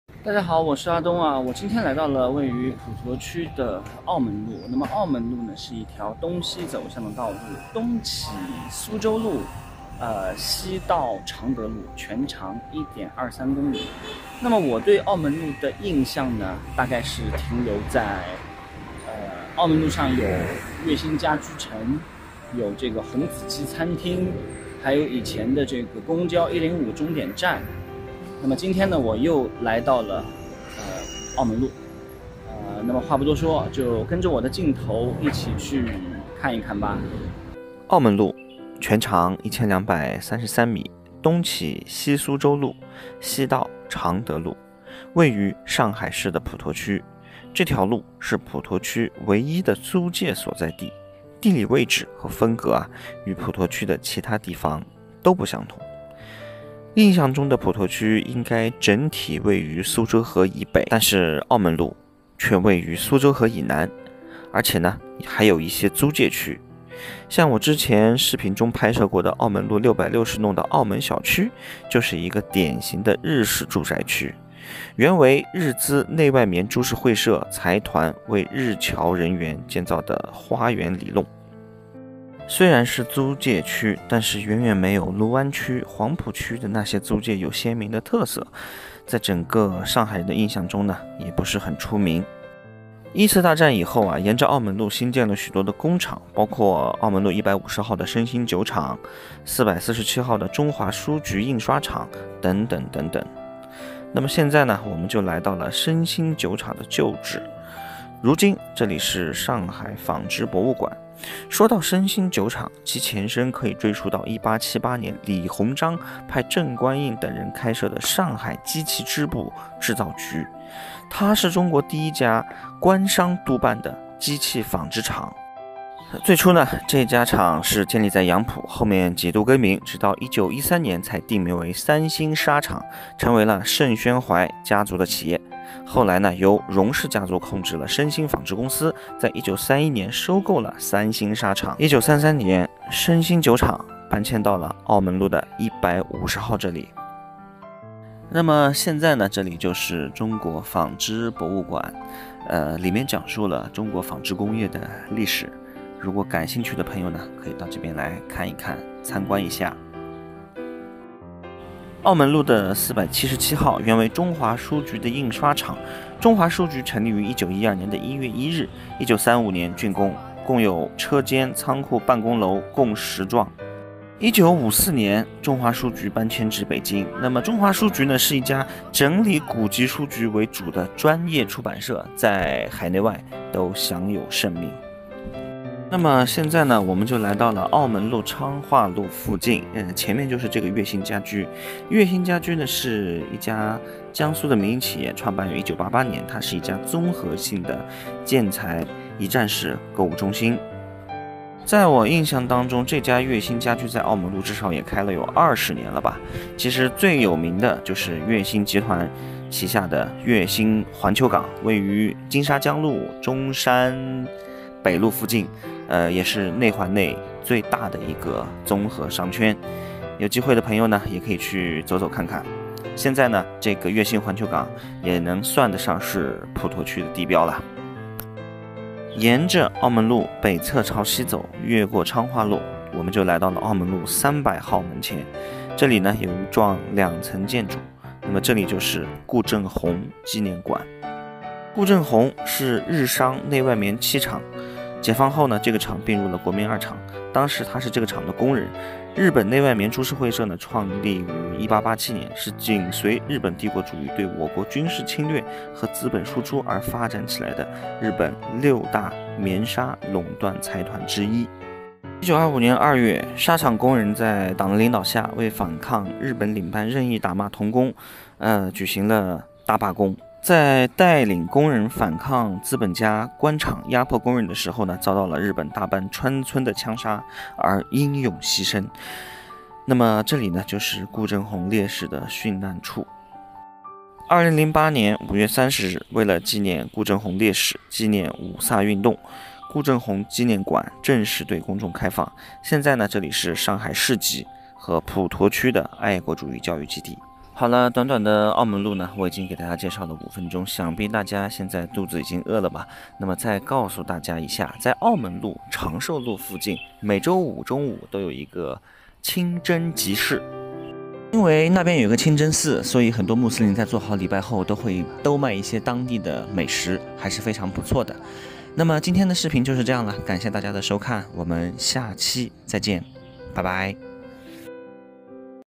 这里以前是上海普陀区的租界区，纺织业很发达，现在发展的如何？
简介:走街串巷用镜头记录历史人文生活的小话唠阿冬，记得关注哦
我会每周更新一到两个影片！